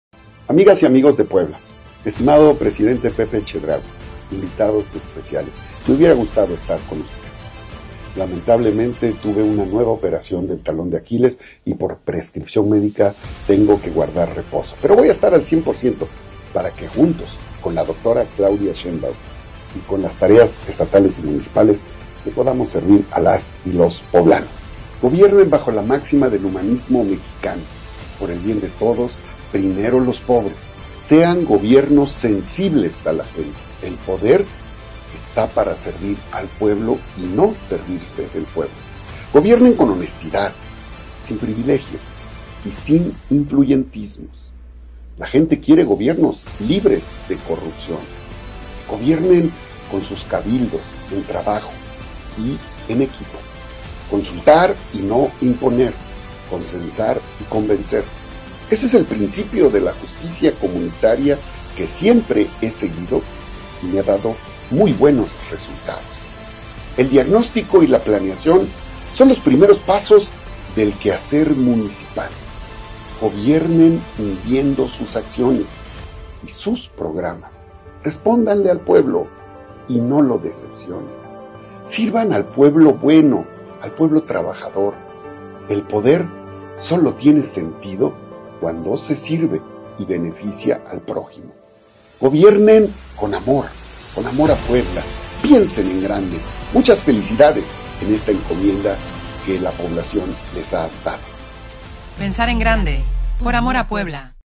Este es el mensaje que Alejandro Armenta envió al presidente municipal Pepe Chedraui
Este es el mensaje que el gobernador electo, Alejandro Armenta envió al presidente municipal Pepe Chedraui.